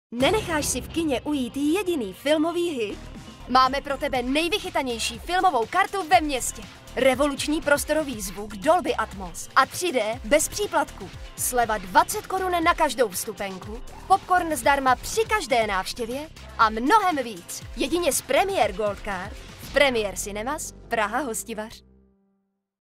Premiere Gold Card - nejvychytanější filmová karta ve městě | Premiere Cinemas
Premiere Gold Card je limitovaná edice filmových karet pro nějvěrnější filmové návštěvníky Premiere Cinemas Praha Hostivař.